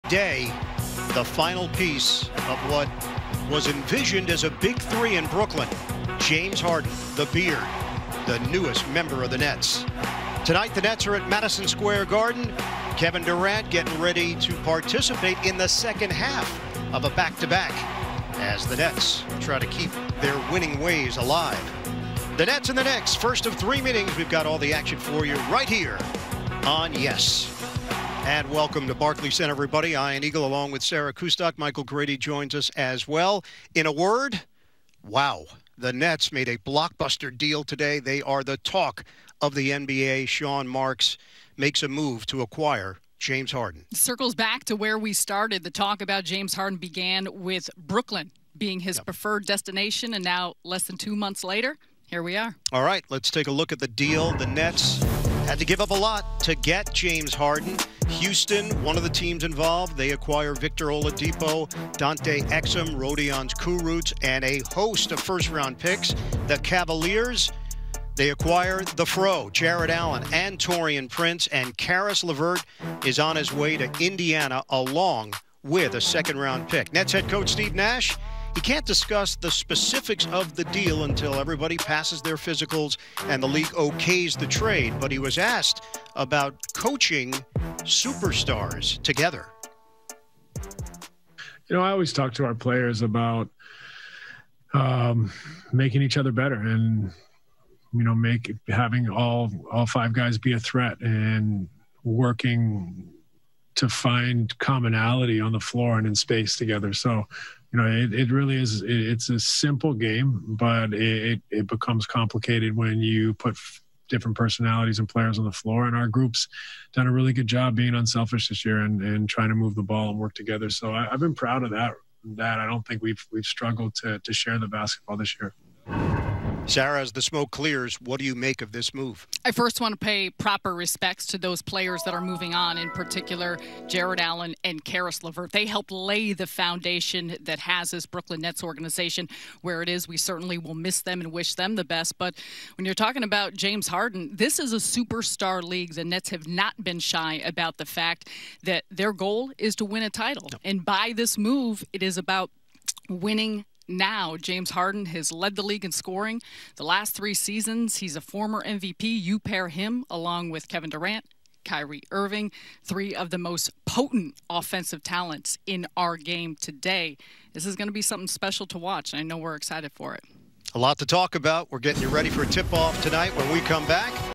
Ian Eagle and Sarah Kustok discuss the reported James Harden blockbuster deal
Ian Eagle and Sarah Kustok share their thoughts on the reported James Harden blockbuster deal.